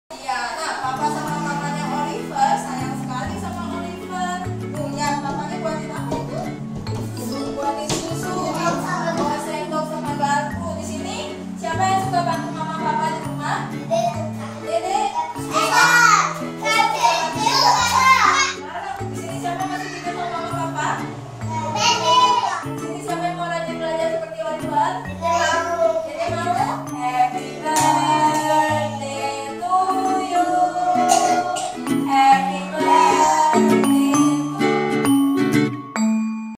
"Happy Birthday" Custom Personalized Pop-Up Birthday Book Gift for Children 02 [ENG SUB] Book G
---"Good story can bring good heart out of a child"---

Available in English and Indonesian Language only
Suitable for kids aged 3-9 years old

=EDUCATIONAL=
Stories of how parents love their child in every growth of the child which helps parents to get closer to child during birthday.

We look forwards to creating a special personalized gift for you.
Thank you so much for taking an interest in our unique book.

Happy Birthday!